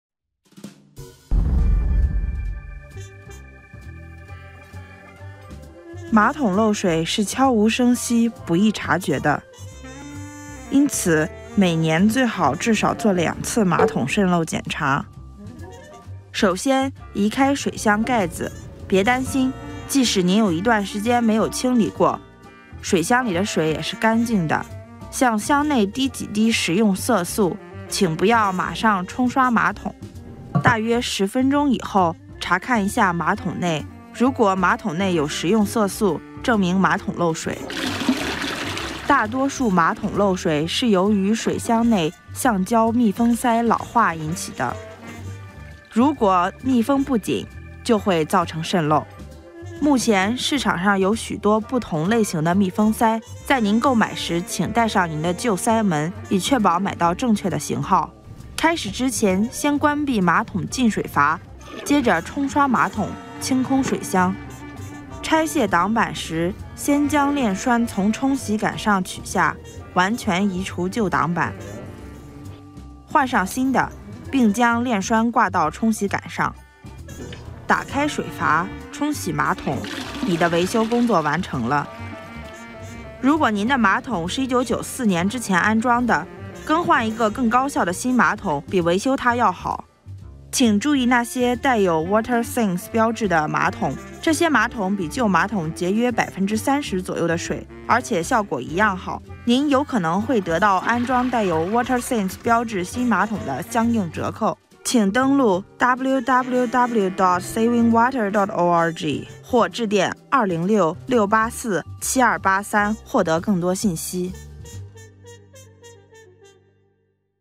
How to Fix a Leaky Toilet (Chinese)
Toilet leaks are sneaky. Many toilet leaks are silent and can occur without you knowing, but fixing them is pretty simple. Chinese translation. Originally produced June 13, 2012.